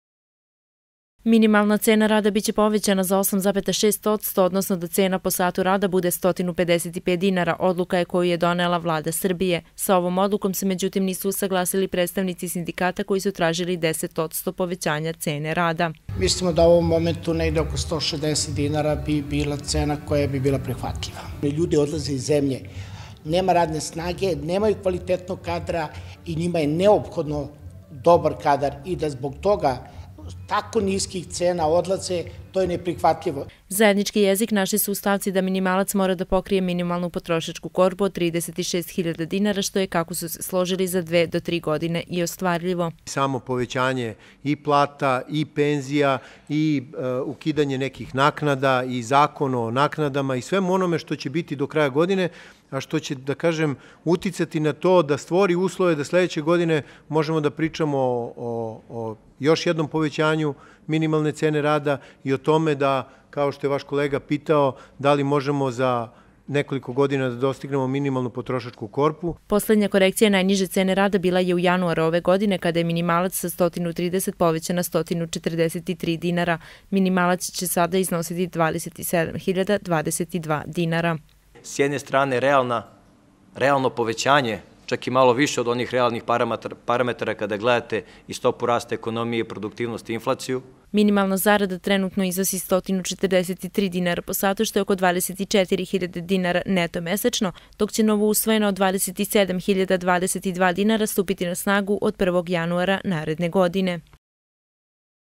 3000 dinara više za minimalac
Мinimalna cena rada biće povećana za 8,6 odsto, odnosno da cena po satu rada bude 155 dinara, odluka je koju je donela Vlada  Srbije. Sa ovom odlukom se, međutim, nisu saglasili predstavnici sindikata koji su  tražili 10 odsto povećanja cene rada.